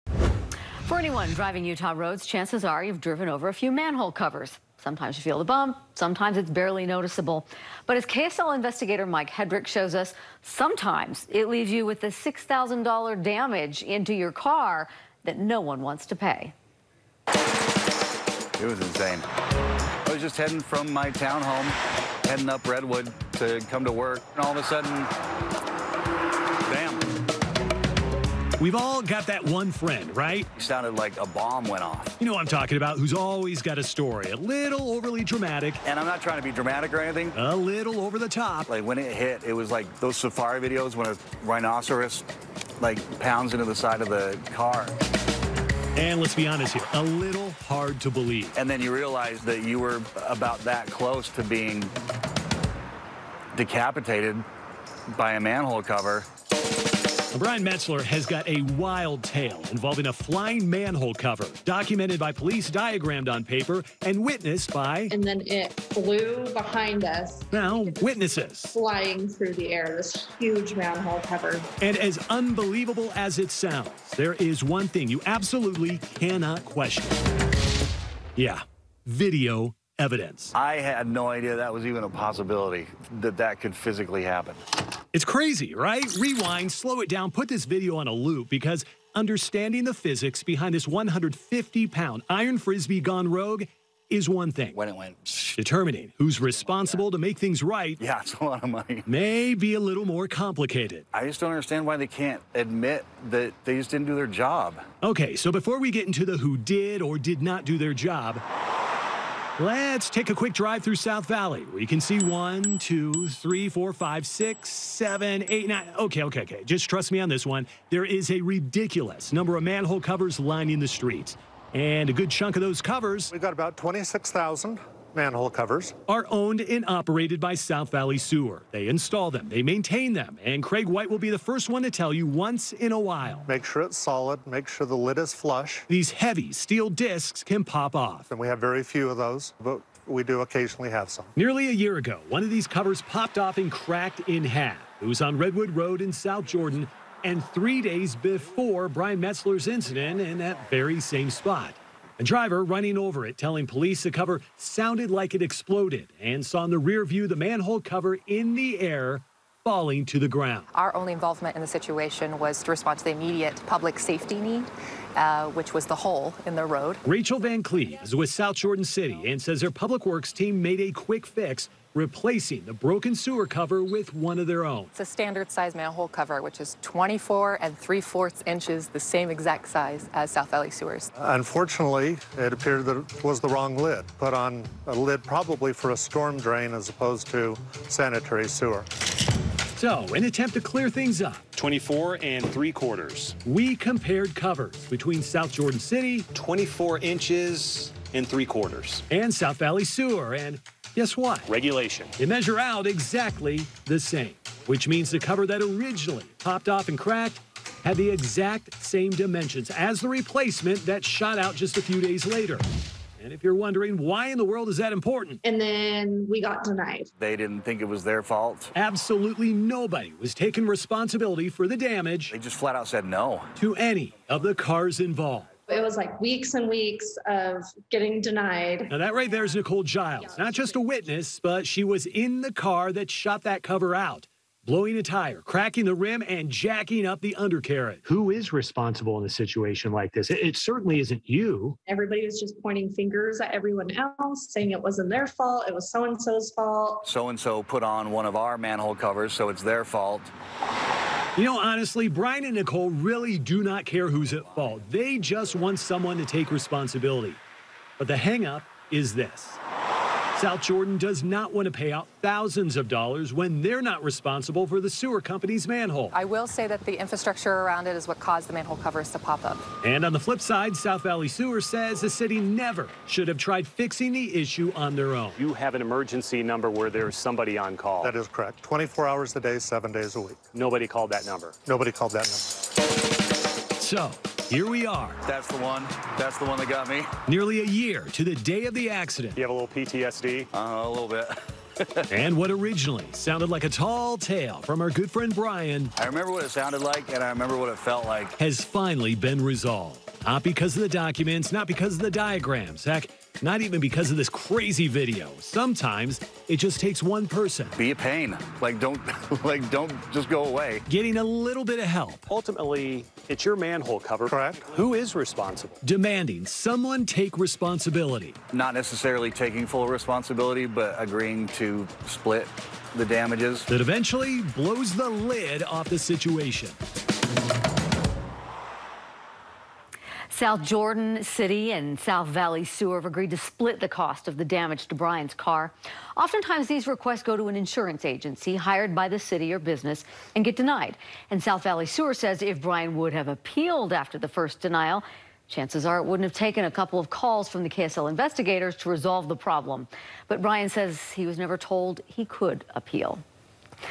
Cutting through the red tape: KSL Investigates a flying road hazard
Last summer, a manhole cover flipped into traffic on Redwood Road in South Jordan, damaging cars. After almost a year of red tape, the drivers contacted KSL Investigates.